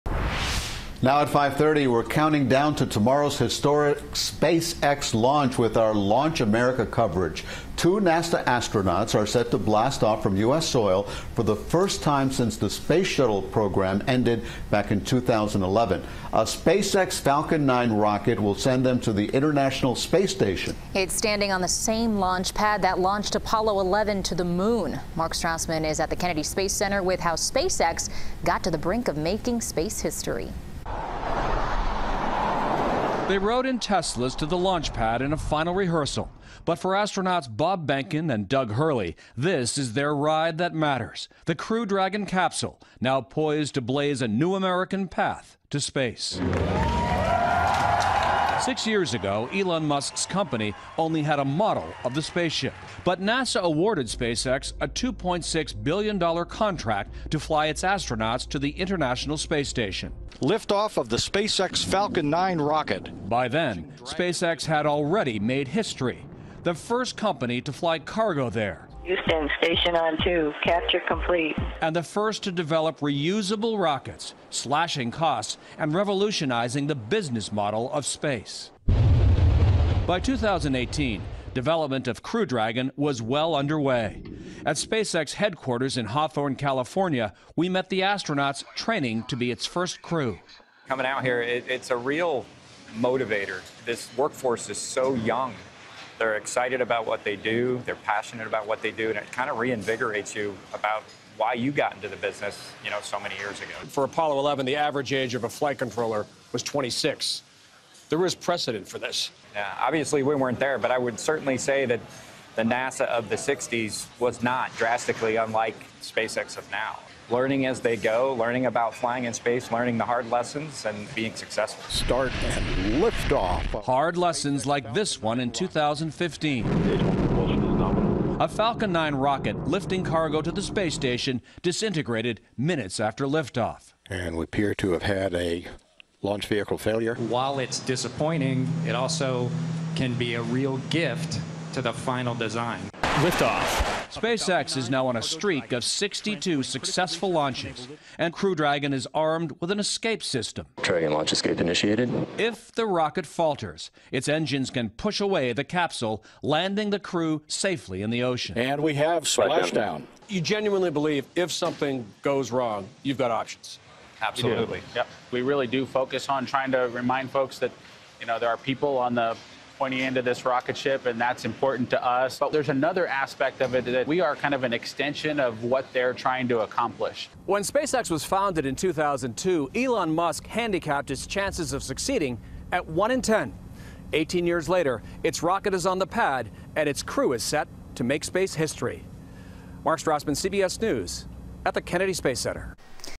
Counting Down To Wednesday's Historic SpaceX Launch
CBS News Correspondent Mark Strassmann reports from Cape Canaveral.